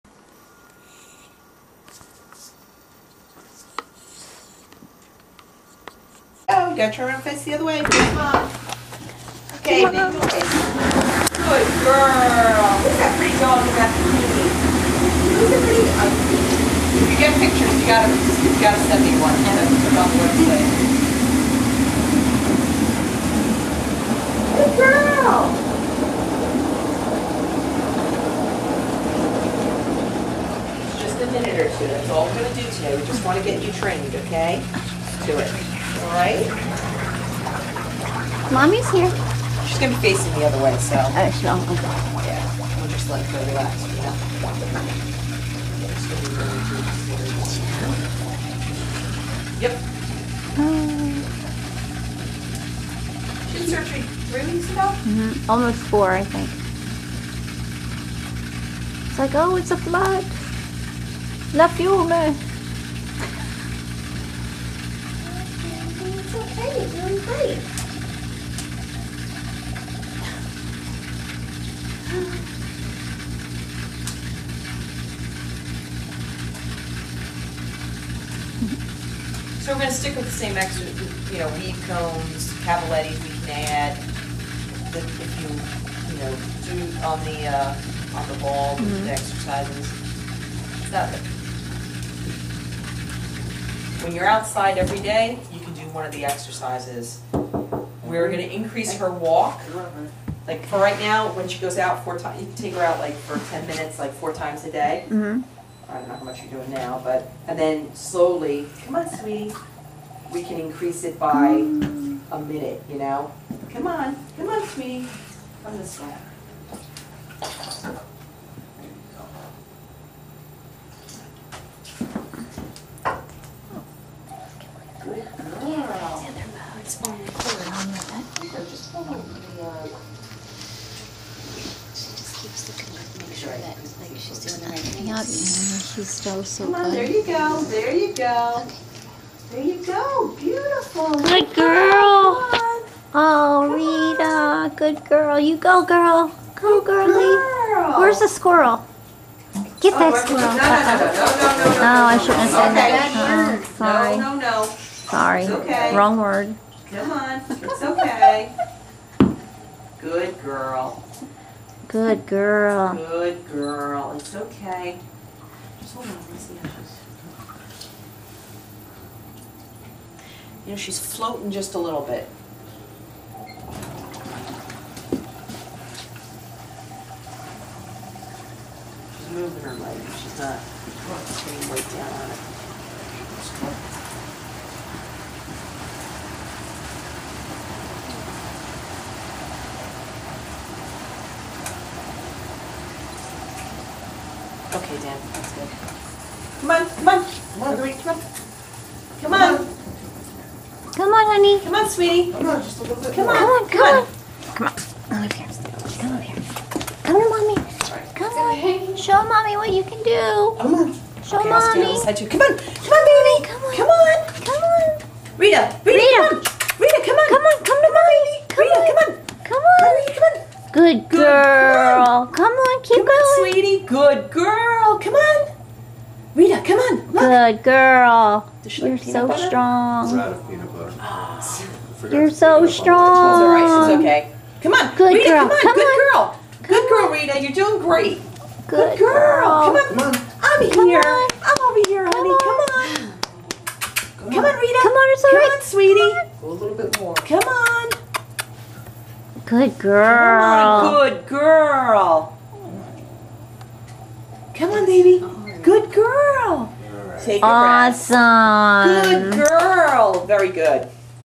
Pet hydrotherapy
my little Westie, Margarita went for her first hydrotherapy treatment post op (Dec 10 was her surgery). she had her ligament replaced with an artificial ligament.She is doing amazing